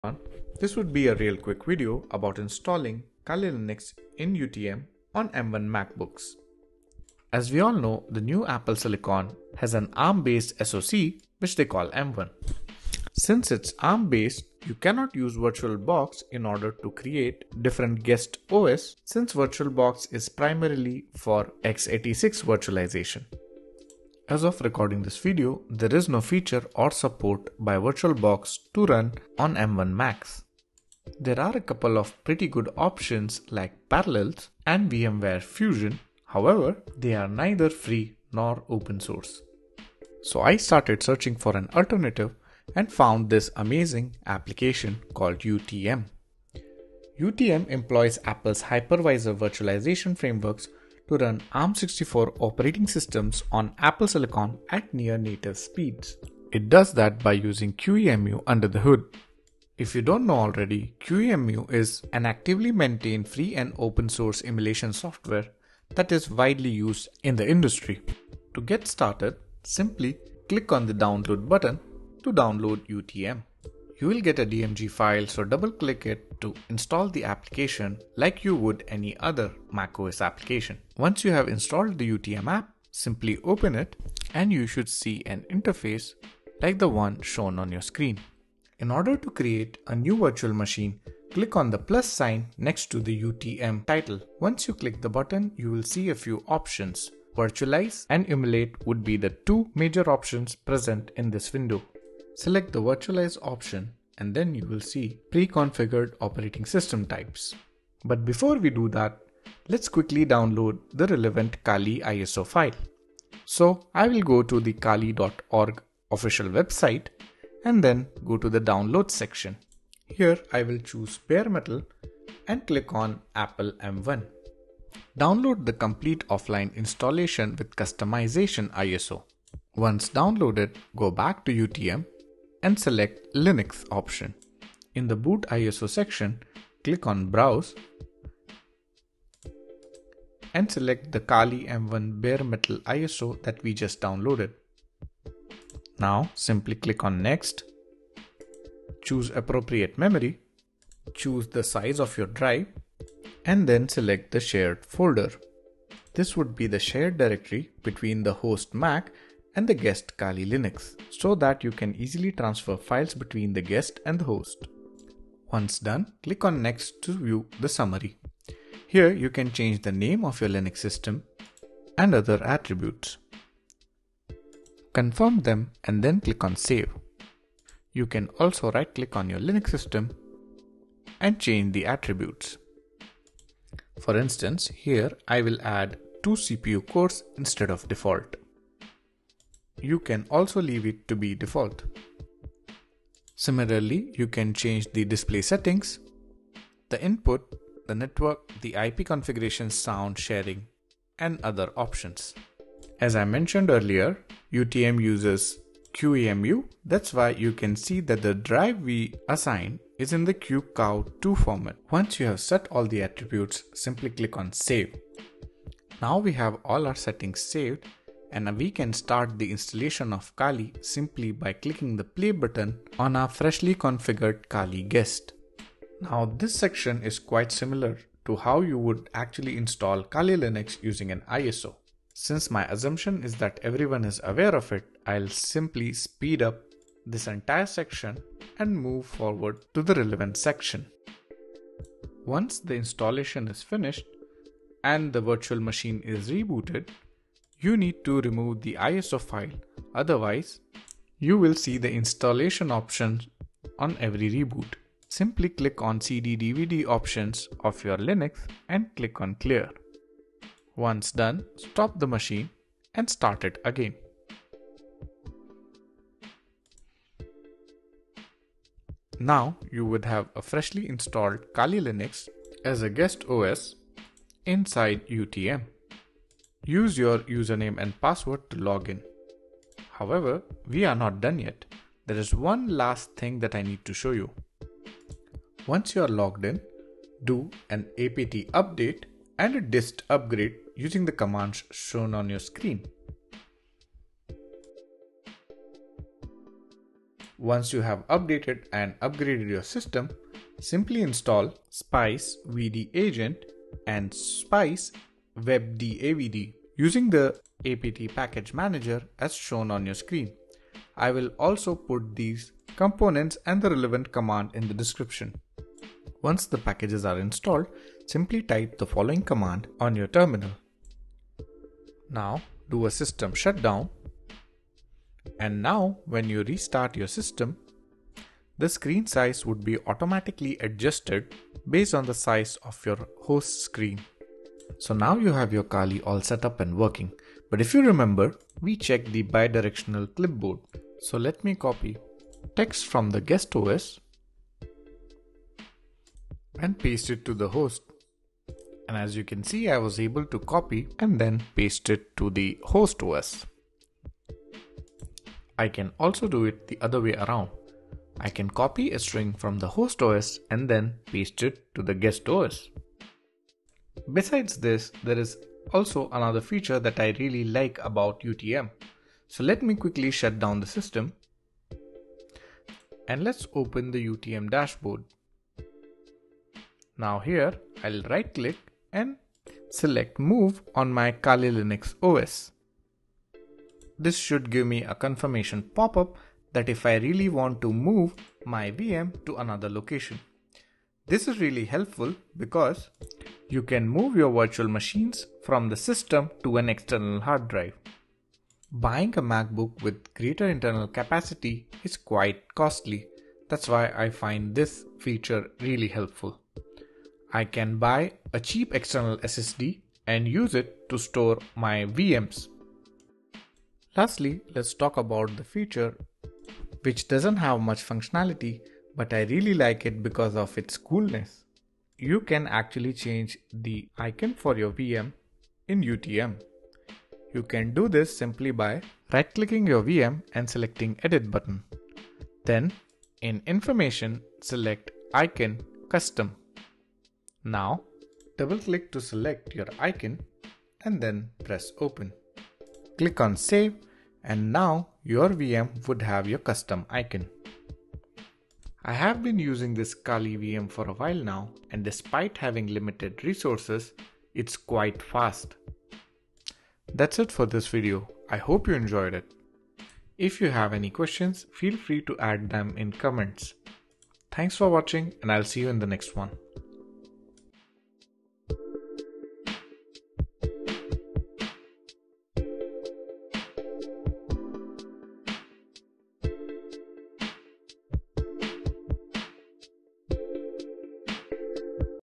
Installing Kali linux on an M1 Macbook via UTM | QEMU linux emulation via UTM on apple silicon
In this video you will learn how to install Kali (or any other OS) as a guest using UTM on an M1 Macbook.

Terminal Commands:
1. Updating the system - `sudo apt update -y && sudo apt dist-upgrade -y`
2. Installing spice packages - `sudo apt install spice-vdagent spice-webdavd`
3. Auto screen resizing - `xrandr --output Virtual-1 --auto`

Video Timestamps:
Introduction - (0:07)
UTM Configuration for VM and Kali Download - (1:30)
OS installation - (3:44)
Removing ISO - (4:08)
Initial login and Package Installation - (4:37)
Screen Resizing - (5:15)
Bidirectional Clipboard Verification - (5:36)
Moving VM to external SSD - (6:16)
Changing Icons - (6:53)
Last thoughts and Outro - (7:28)

github/twitter : nigamelastic